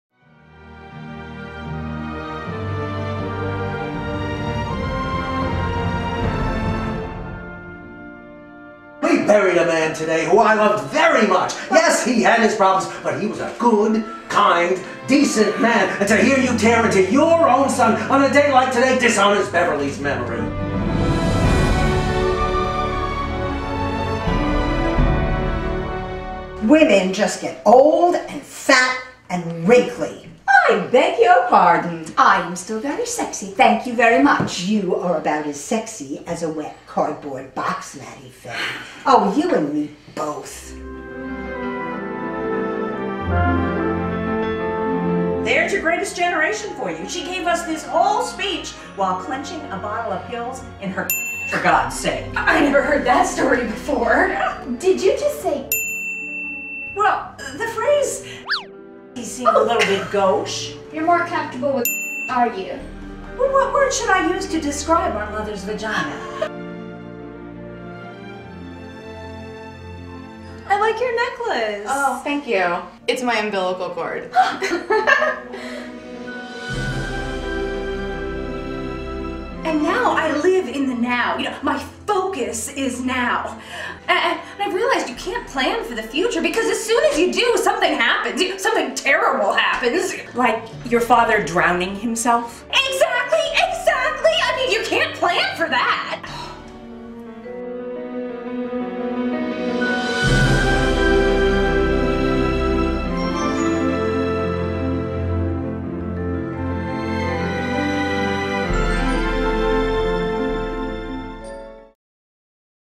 August Osage County First Trailer1
Trailer for PinnWorth Productions' August Osage County, opening at Kelsey Theatre in West Windsor, NJ on January 8th, 2016